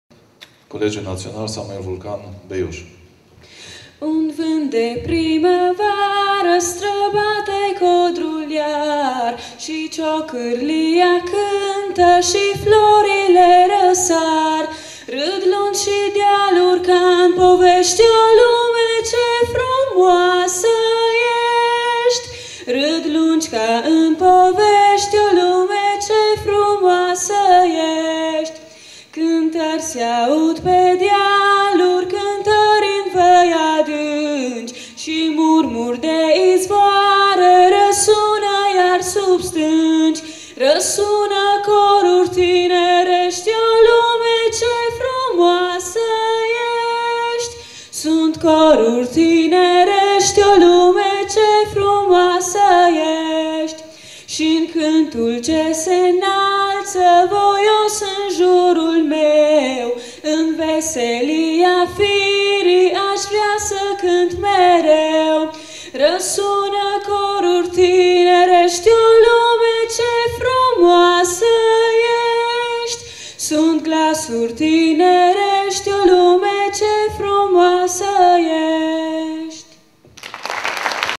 Colegiul Național SAMUIL VULCAN, Beiuș
Festivalul "Să-l cunoaștem pe Beethoven!", ediția a VIII-a, 5 noiembrie 2015